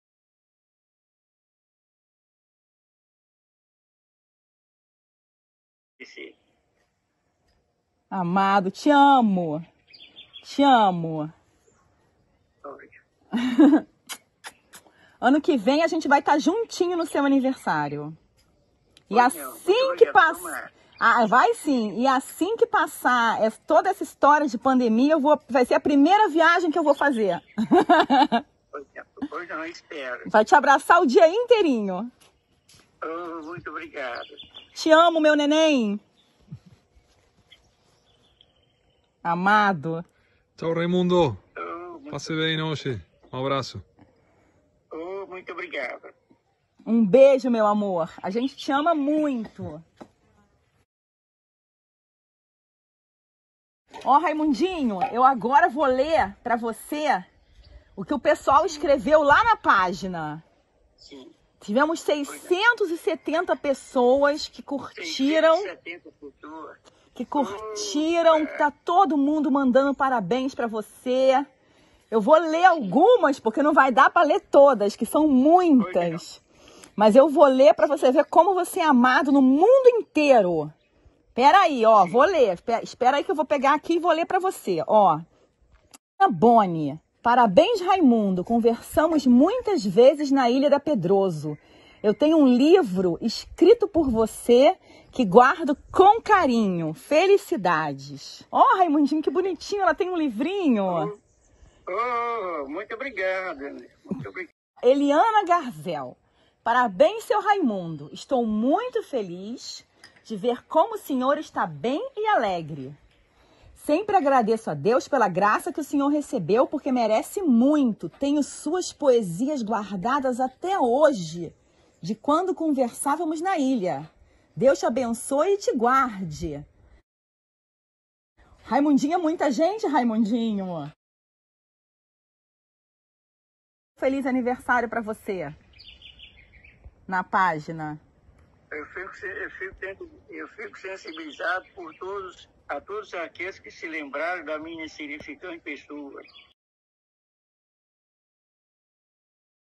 Feliz 82 anos Raimundinho! Happy Birthday!
Olá pessoal, sejam muito bem vindos por aqui!

No dia 01 de agosto Raimundo Arruda Sobrinho, completou 82 anos!

Como de costume desde que nos conhecemos em 2011 celebramos seu aniversário, mas desta vez por conta da pandemia, foi virtualmente!

Este vídeo é um compilado da nossa celebração deste ano, na qual eu li algumas mensagens que os amigos postaram na página do Facebook dele! Vejam até o final para verem o agradecimento dele aos amigos que o felicitaram na página.

Beijos com amor,

Shalla

ENGLISH VERSION:

Hello guys, welcome here!

On August 1st Raimundo Arruda Sobrinho turned 82!

As usual, since we met in 2011, we have celebrated his birthday, but this time because of the pandemic, it was virtually!

This video is a compilation of our celebration this year, in which I read some messages that friends posted on his Facebook page! Please see until the end when his thanks to the friends who congratulated him on the page.

Kisses with love,

Shalla